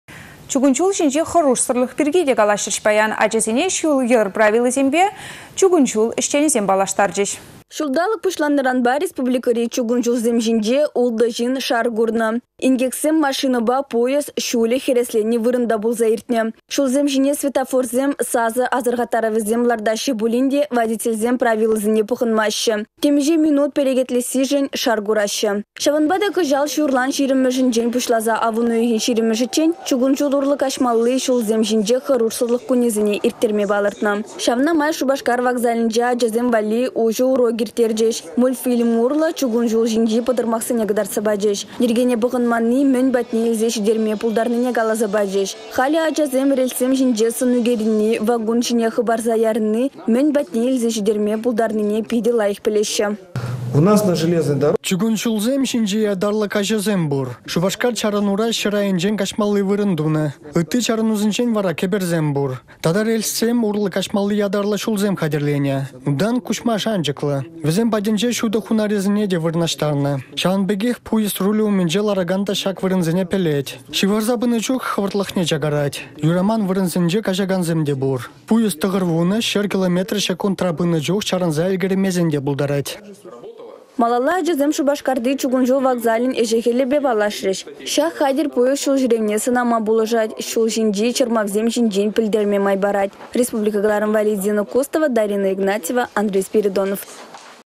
Чукун çул урлă каçнă чухне асăрхануллă пулмалла
Чукун çул çинчи хăрушсăрлăх пирки те калаçрĕç паян. Ачасене çул-йĕр правилисемпе ашшĕ-амăшĕ паллаштарать пулин те чукун çул ĕçченĕсем те айккинче юлмаççĕ.